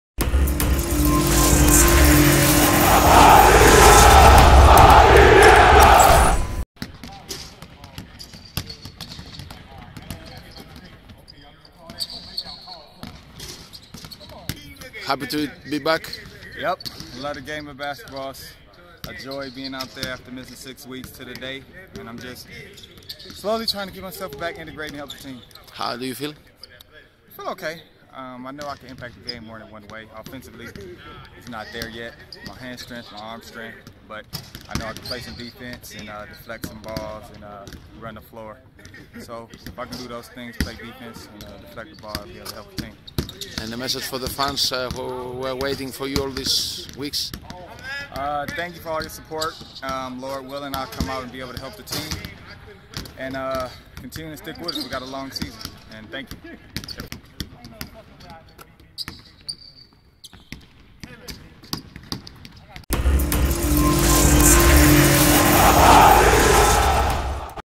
Ο Τρεμέλ Ντάρντεν μιλά για την επιστροφή του | Tremmell Darden on his comeback
Ο Αμερικανός μίλησε στην κάμερα του Olympiacos BC TV για την επιστροφή του σε αγωνιστική δράση ύστερα από απουσία έξι εβδομάδων λόγω του τραυματισμού του στο χέρι.

Tremmell Darden spoke to Olympiacos BC TV about his come back after his 6 week absence due to an arm injury